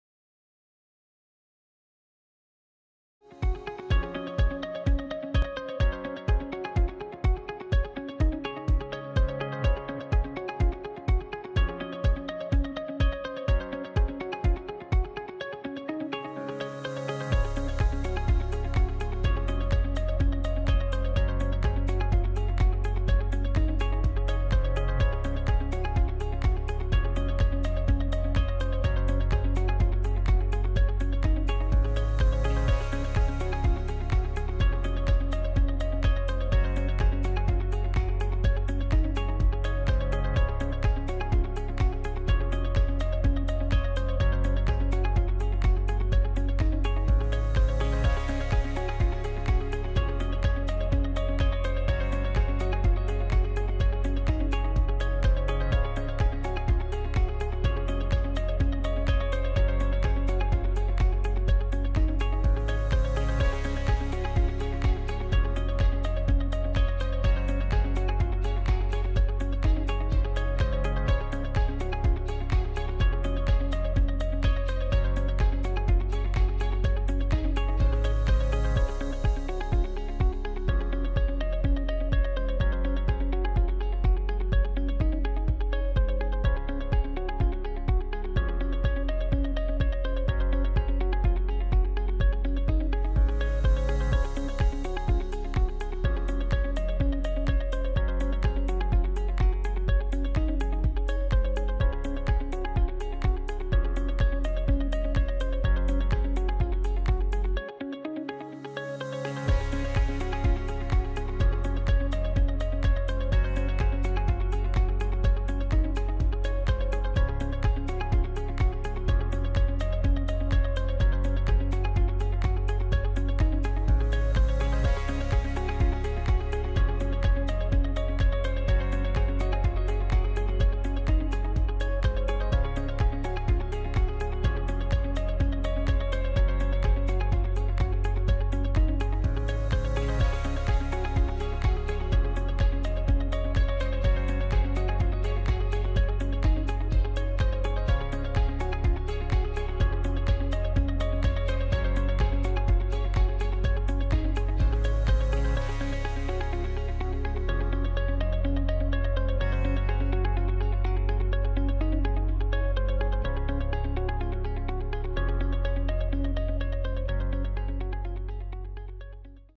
Drawing Timelapse Ancestral Boomerang
Abstract digital pencil drawing. Done in iPad Pro, using Adobe Fresco